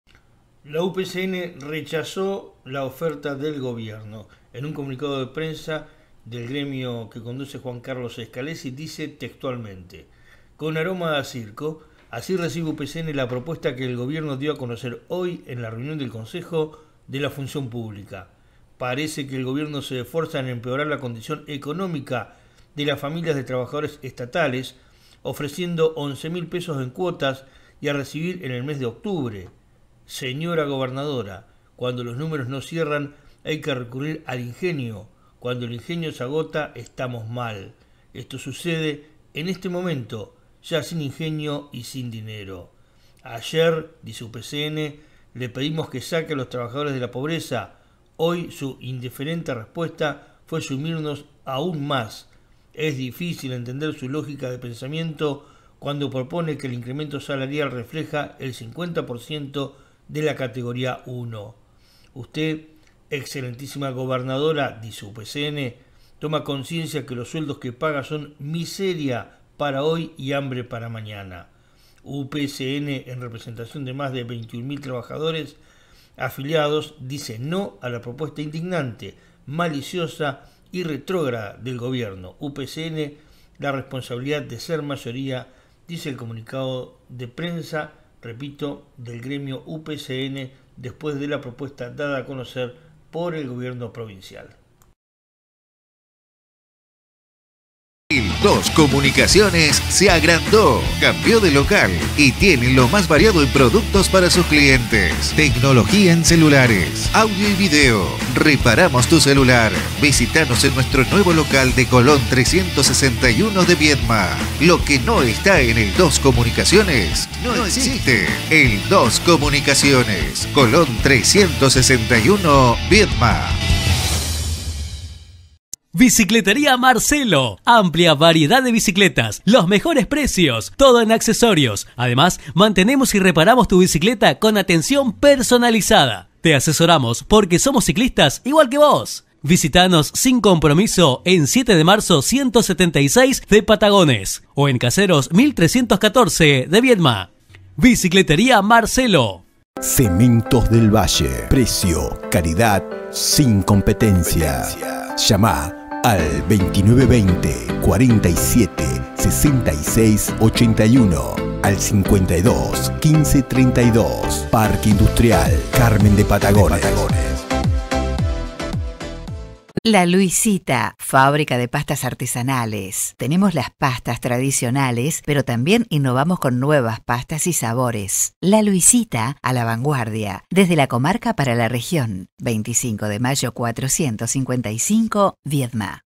PARA UPCN LA PROPUESTA DEL GOBIERNO ES INDIGNANTE, MALICIOSA Y RETROGRADA
UPCN dice NO!!
Con aroma a circo, así recibe UPCN la propuesta que el gobierno dio a conocer hoy en la reunión del Consejo de la Función Pública.
Parece que el gobierno se esfuerza en empeorar la condición económica de las familias de trabajadores estatales, ofreciendo $11000 en cuotas y a recibir en el mes de octubre.
Sra. gobernadora cuando los números no cierran hay que recurrir al ingenio, cuando el ingenio se agota estamos mal, esto sucede en este momento ya sin ingenio y sin dinero.
Ayer le pedimos que saque a los trabajadores de la pobreza, hoy su indiferente respuesta fue sumirnos aún más; es difícil entender su lógica de pensamiento cuando propone que el incremento salarial refleja el 50% de la categoría 1.
Usted excelentísima Gobernadora…. toma conciencia que los sueldos que paga son miseria para hoy y hambre para mañana?